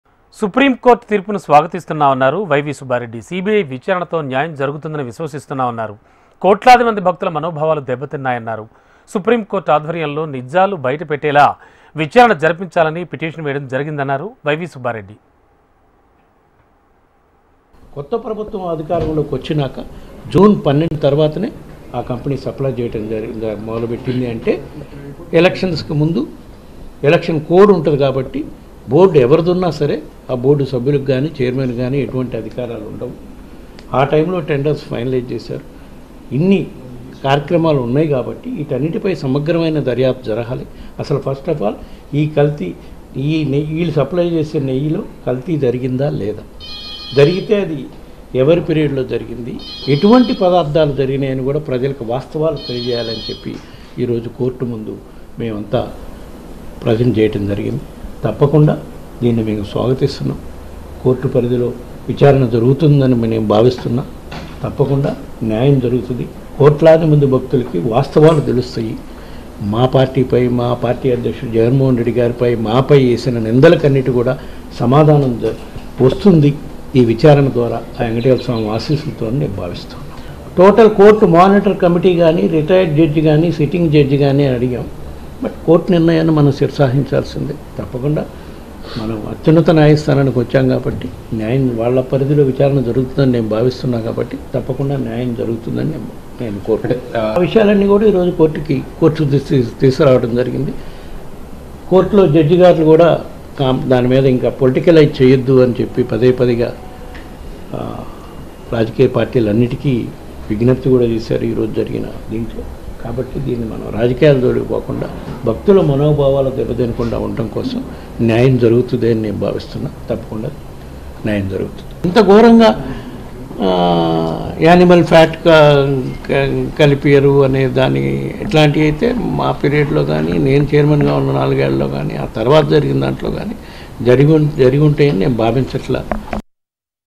తిరుపతి లడ్డు కల్తీ పై న్యాయం గెలిచింది ..! | YV Subba Reddy Reaction On supreme court judgement
తిరుపతి లడ్డు కల్తీ పై న్యాయం గెలిచింది ..! | YV Subba Reddy Reaction On supreme court judgement | hmtv